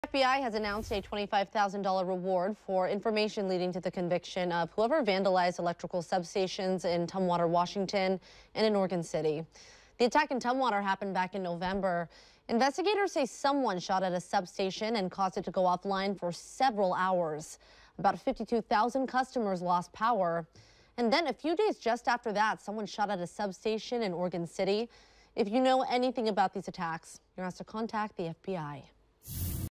$25K rewards offered for information about Oregon, Washington substation shootings
The FBI has announced rewards for information leading to the conviction of the people responsible for vandalizing electrical substations in Tumwater, Washington, and Oregon City last year.